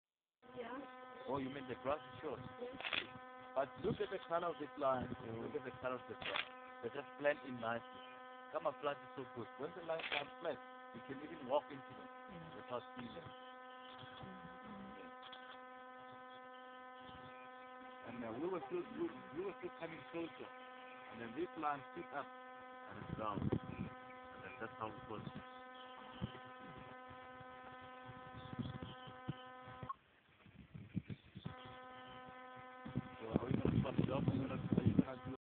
Leoas na savana
Safari no Kapama Private Reserve em Hoedspruit, provincia de Limpopo na Africa do Sul, reserva particular para safari na savana sul africana.